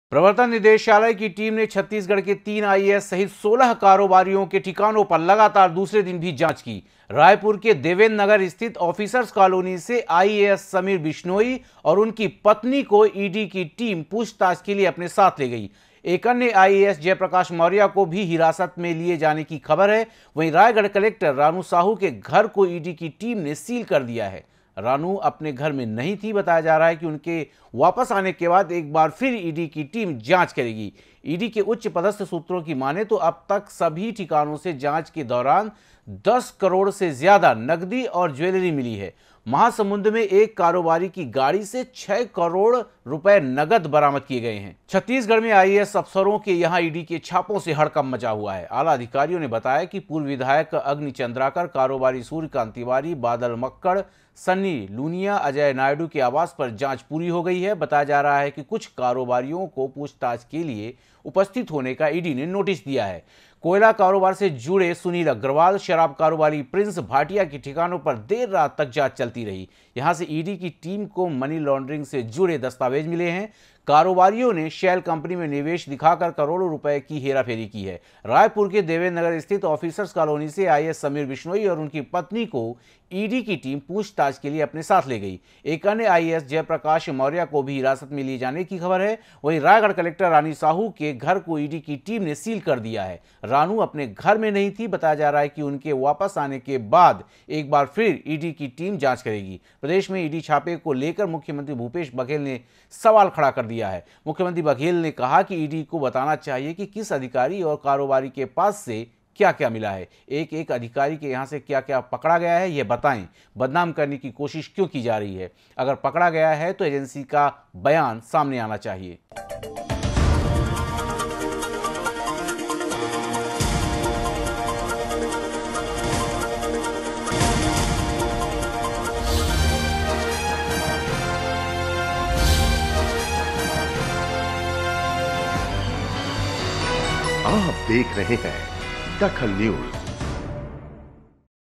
DAKHAL NEWS छत्‍तीसगढ़ में ईडी की कार्रवाई जारी
छत्‍तीसगढ़ में ईडी की कार्रवाई जारी
आइएएस मौर्या, बिश्नोई व उनकी पत्नी हिरासत में
रायगढ़ कलेक्टर का बंगला सील